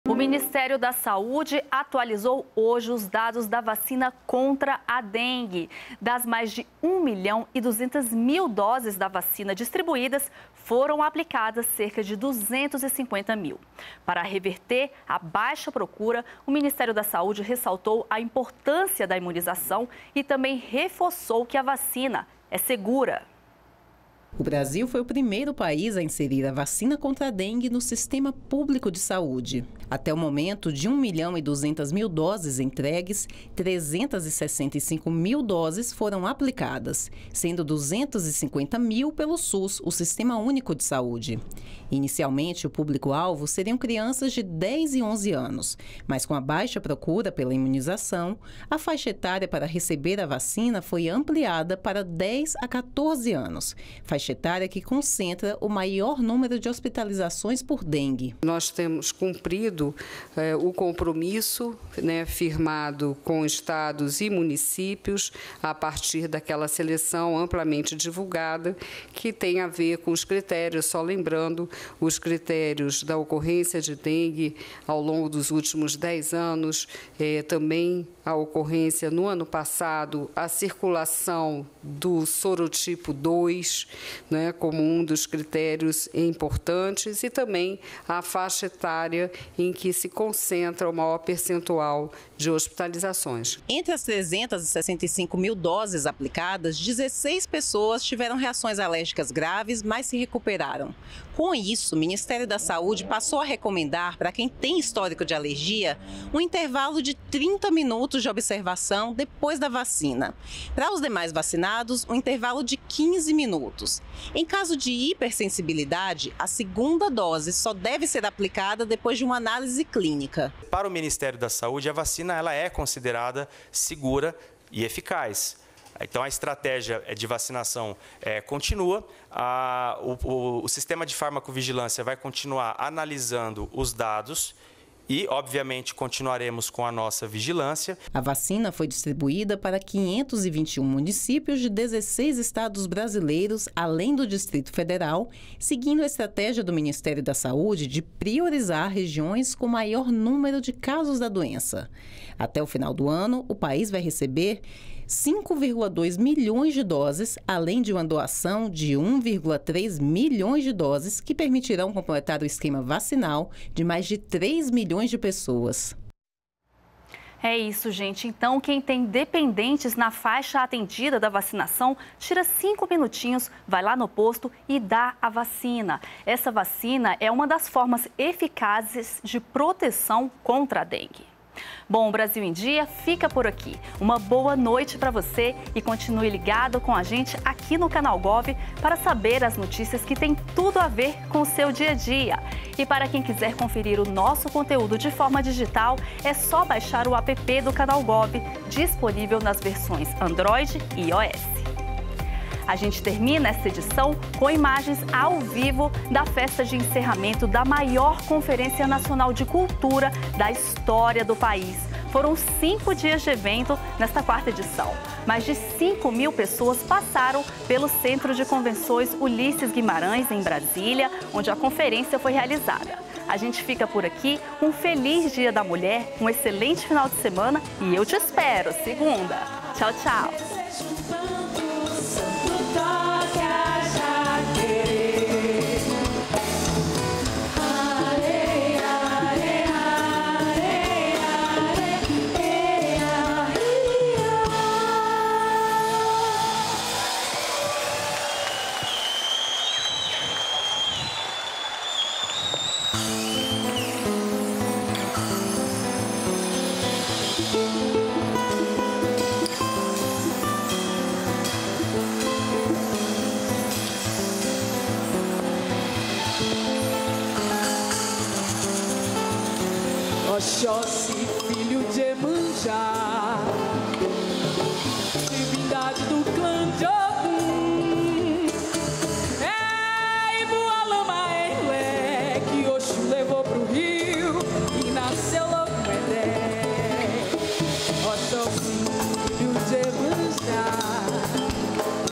MINISTÉRIO DA SAÚDE REITERA SEGURANÇA DA VACINA CONTRA A DENGUE
O Ministério da Saúde atualizou os dados e reiterou a segurança e a importância da vacina contra a dengue. Até sexta-feira (8), 365 mil doses haviam sido aplicadas, sendo 250 mil pelo SUS. A faixa etária para receber a vacina foi ampliada para de 10 a 14 anos.